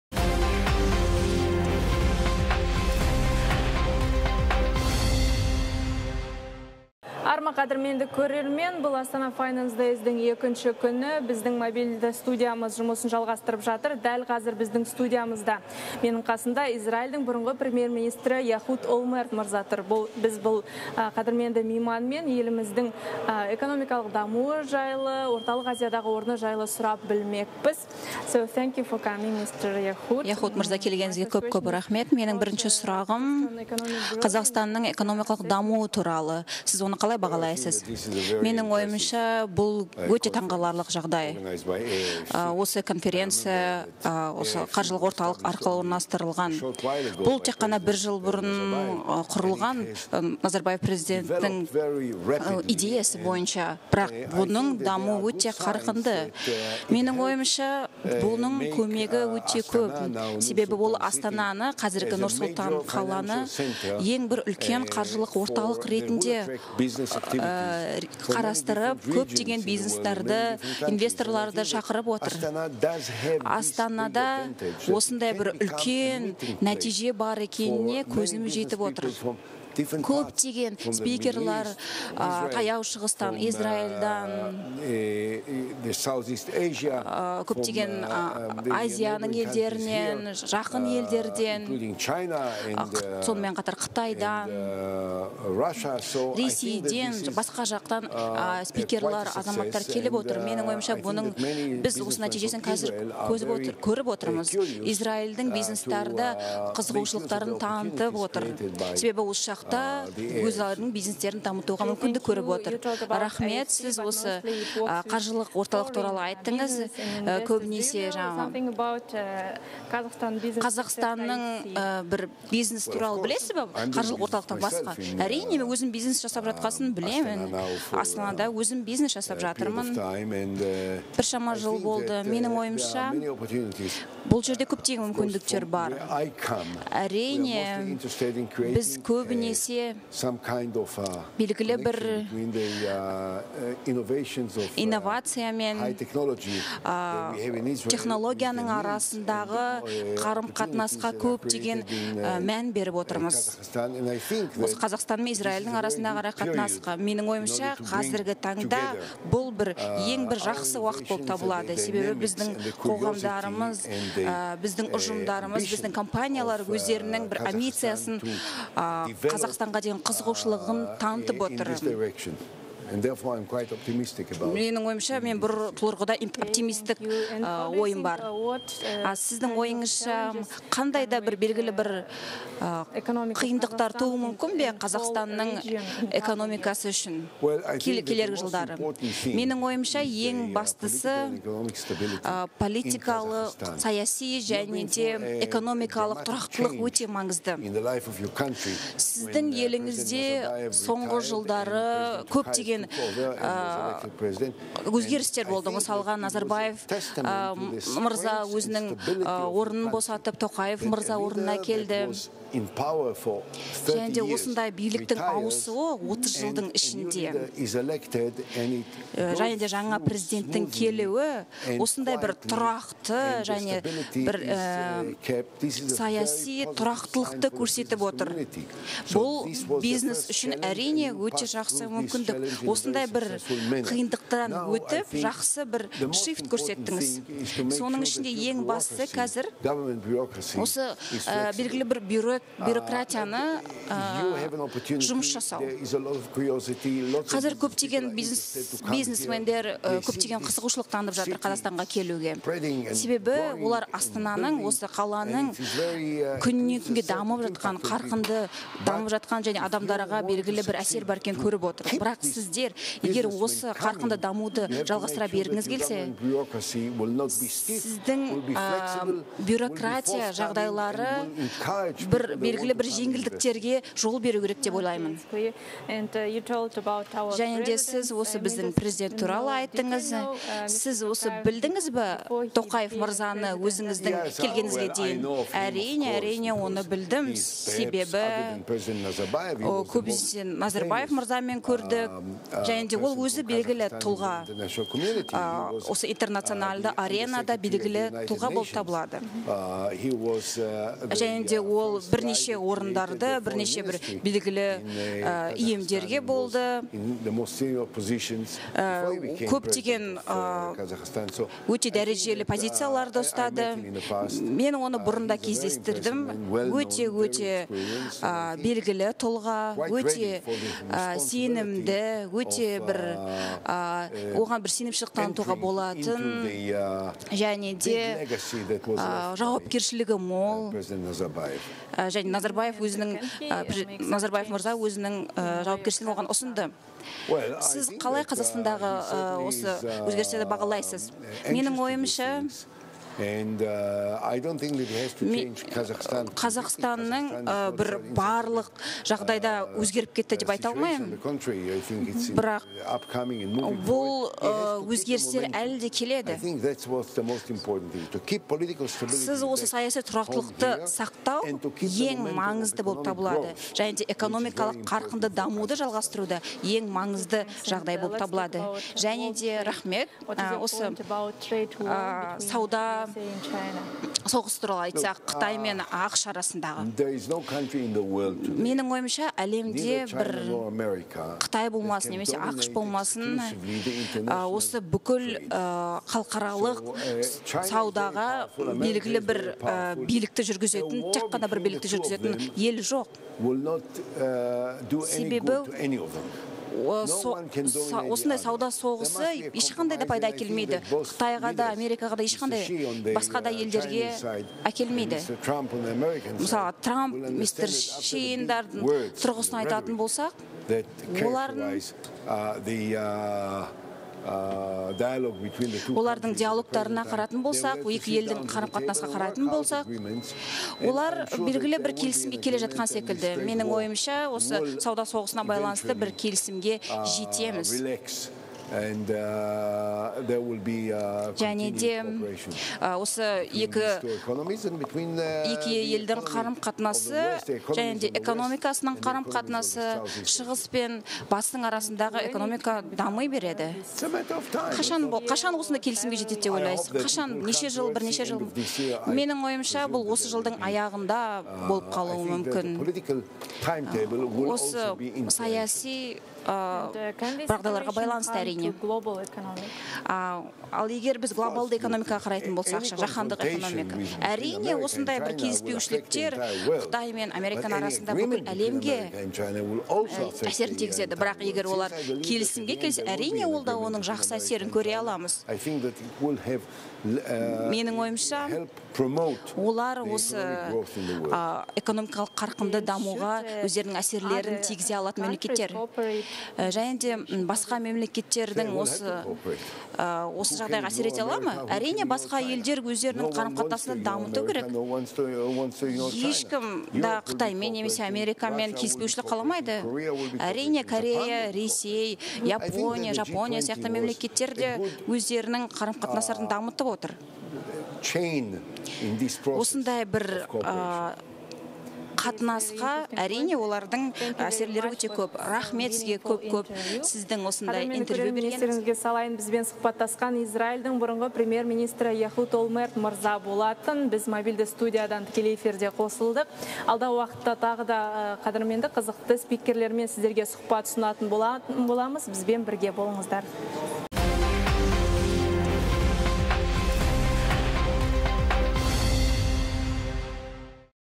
Яхуд Олмертпен сұхбат/LiveStream (02.07.2019)
Тақырып: Жаһандық экономика нарығындағы үрдіс.

Бағдарлама қонағы: Израильдің бұрыңғы премьер-министрі - Яхуд Олмерт.

Тілші: Кәусар Аяпберген.